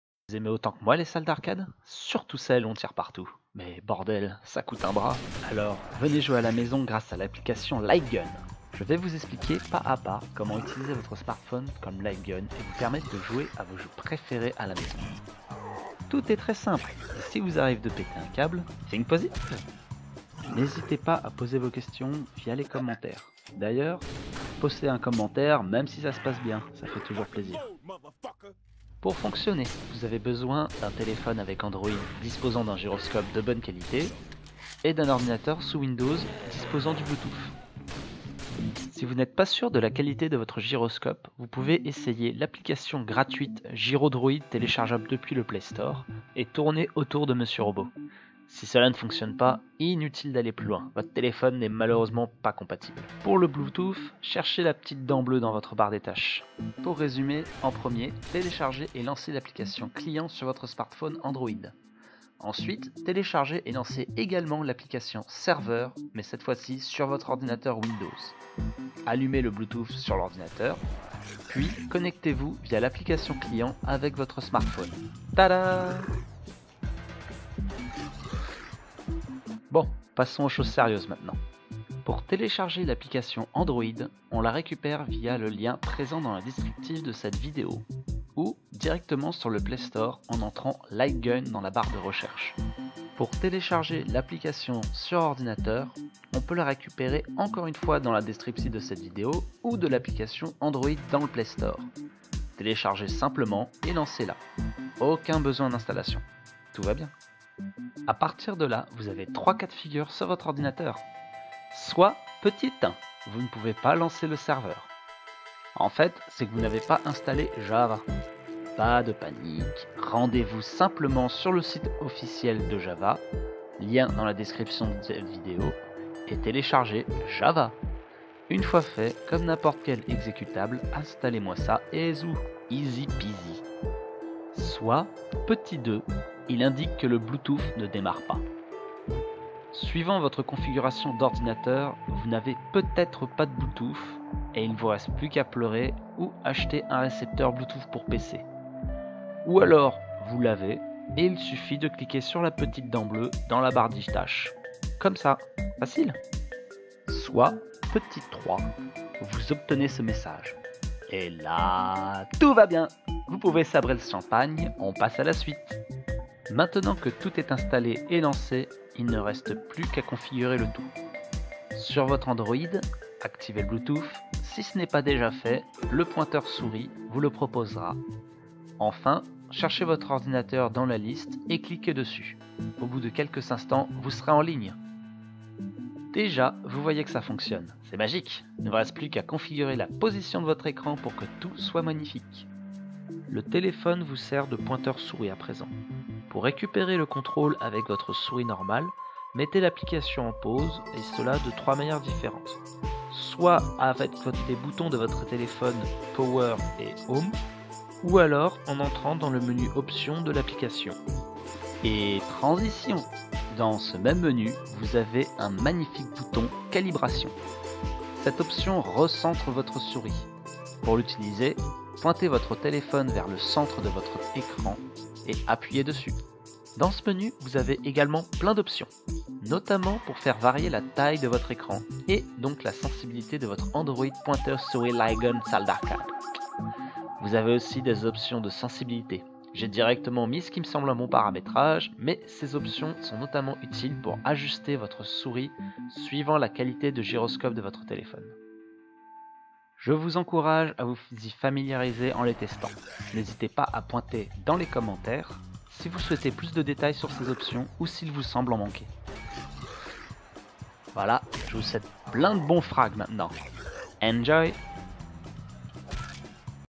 Pointer Souris Light Gun Android Tutoriel
This is the tutoriel to use the Android Light Gun application
With this app you can play games and use your phone as a Light Gun, or a Track Pad, or a Graphics Tablet